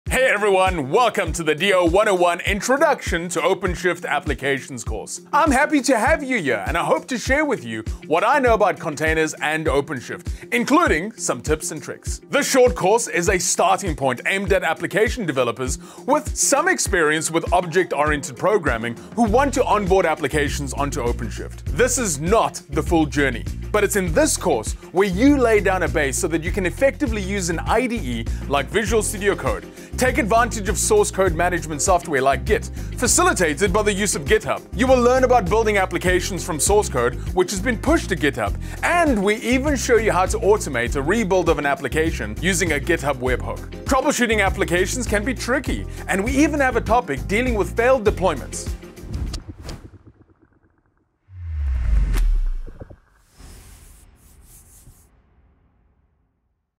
Introduction to Red Hat OpenShift Applications (DO101)
Hear from the instructor of Introduction to Red Hat OpenShift Applications (DO101) about what you'll learn in this developer focused course.